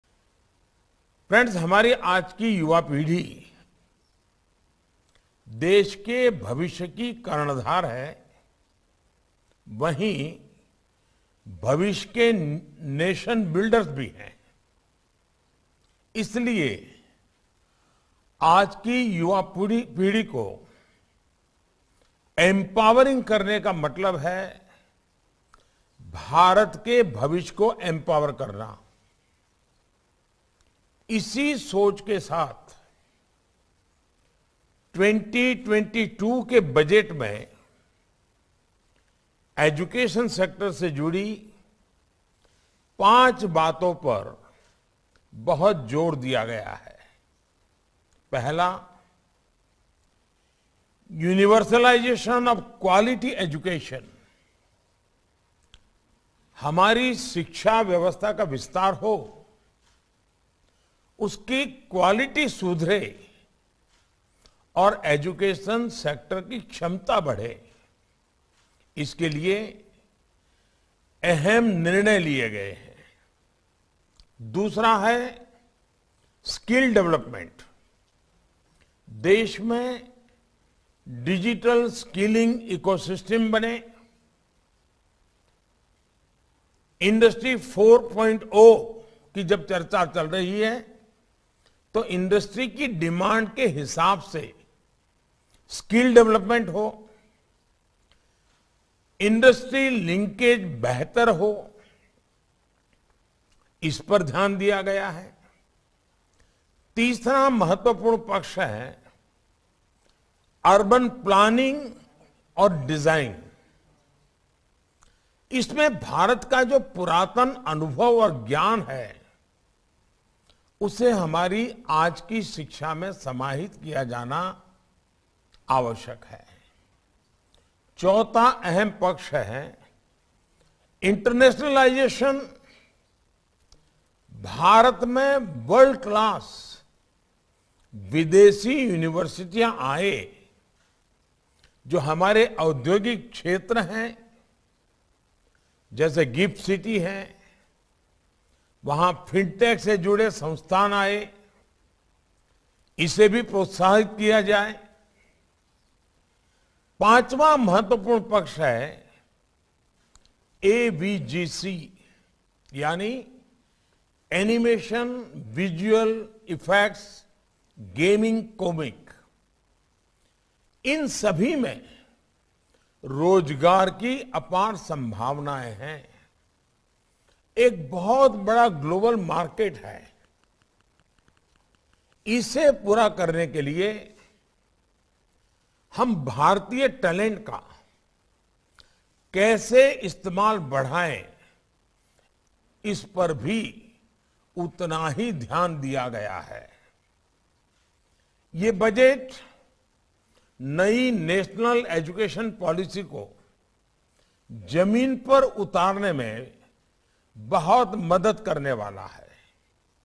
Budget 2022 focuses on five key aspects pertaining to education sector…Know all details here!
PM Narendra Modi addressed a webinar on positive impact of Union Budget 2022 on the education and skill sectors. The Prime Minister emphasized the importance of young generation in the process of nation building. He said, “Empowering our youth who are future nation builder, is empowering India’s future.”